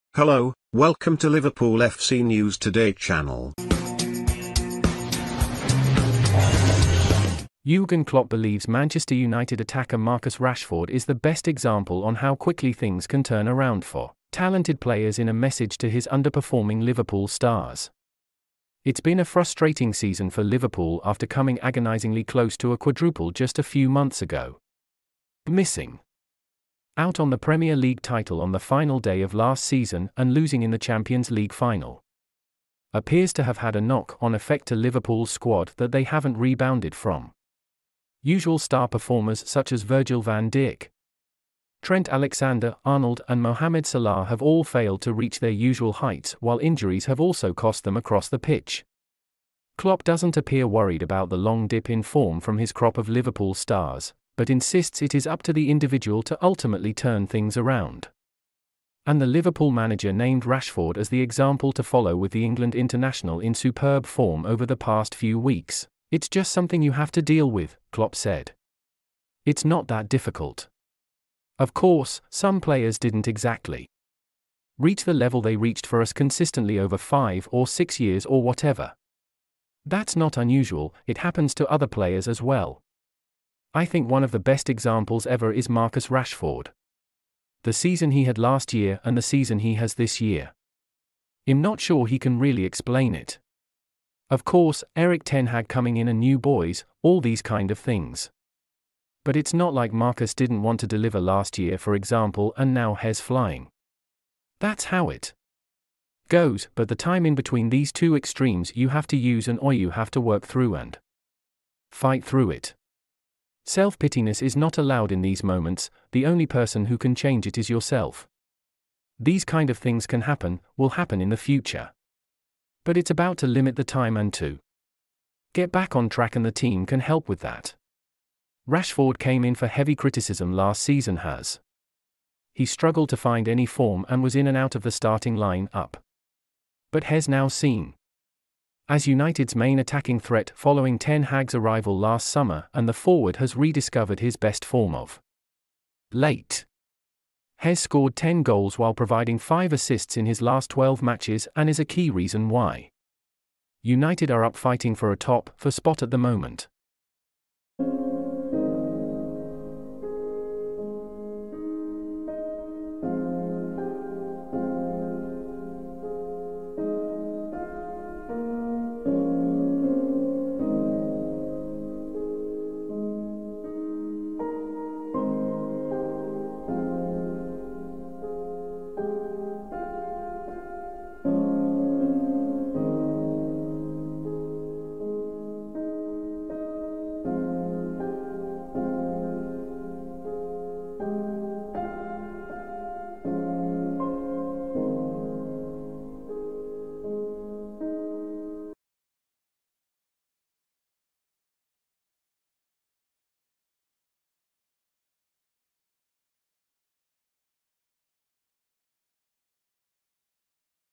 Jurgen Klopp names Marcus Rashford as 'best example' for underperforming Liverpool stars - news...
Jurgen Klopp names Marcus Rashford as 'best example' for underperforming Liverpool stars - news today

Jurgen Klopp believes Manchester United attacker Marcus Rashford is the 'best example' on how quickly things can turn around for talented players in a message to his underperforming Liverpool stars. It's been a frustrating season for Liverpool after coming agonisingly close to a quadruple just a...